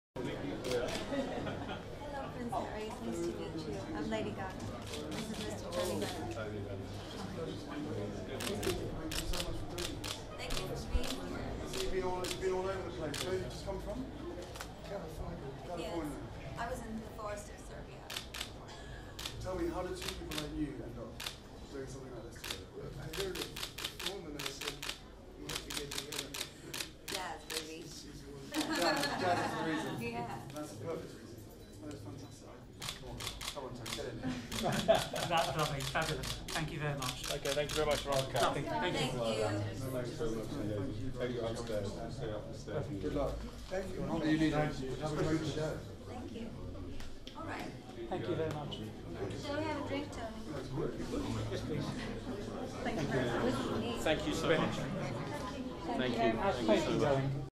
When Harry met Gaga! Royal prince meets pop royalty
When Harry met Gaga! Royal prince meets pop royalty in London for WellChild event.